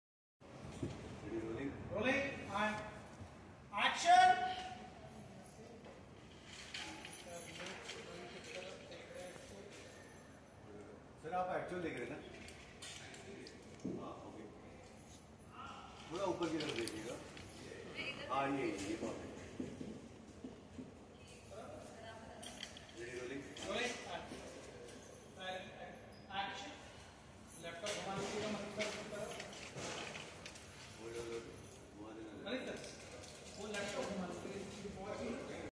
serial ki shooting। behind the scene of serial।bts
ye rishta kya kehlata hai BTS shoot। yrkkh behind the scene ।  serial ki shooting kaise hoti hai।